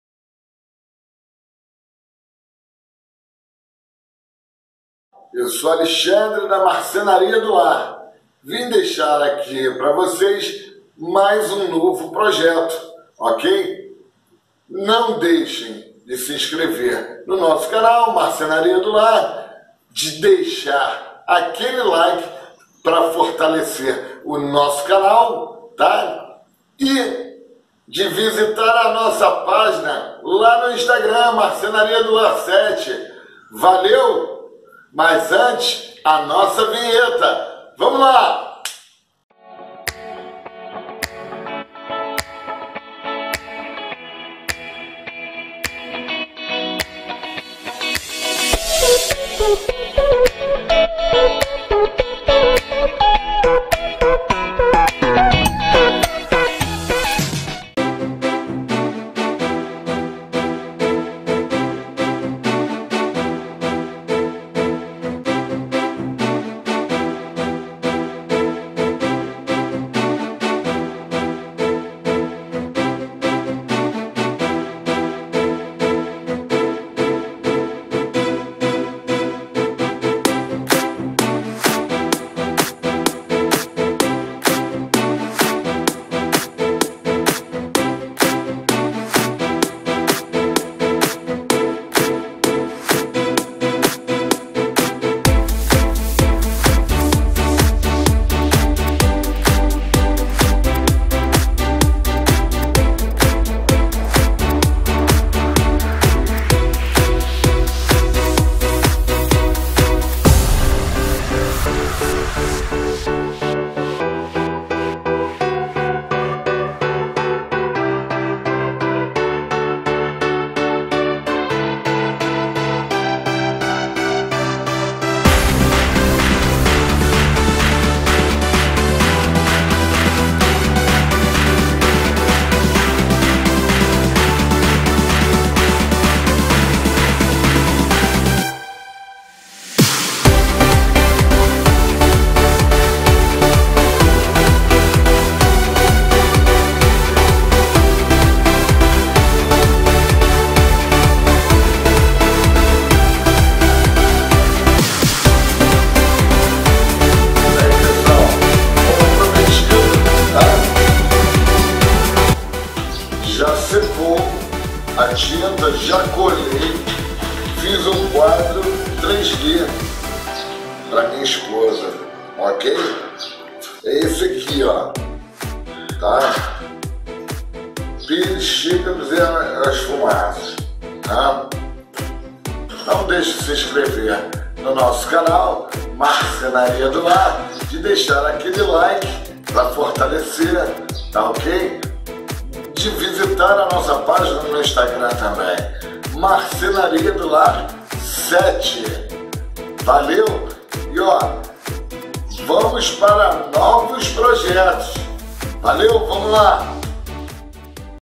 APRENDA A FAZER QUADRO 3D DECORAÇÃO CAFÉ !!! LEARN TO MAKE FRAME 3D DECOR CAFE !!!
LEARN TO MAKE FRAME 3D DECOR CAFE !!!
MATERIAL UTILIZADO:

*UM RETALHO DE TÁBUA DE PINUS
*COLA ESTANTÂNEA
*TINTA BRANCA SPRAY
*VERNIZ TRANSPARENTE SPRAY

FERRAMENTAS UTILIZADAS:

*SERRA TICO-TICO
*LIXADEIRA

MATERIAL USED:

* A RULE OF PINUS TABLET
* STAINED GLASS
* WHITE INK SPRAY
* TRANSPARENT VERNIZ SPRAY

TOOLS USED:

* TICO-TICO SAW
* SANDER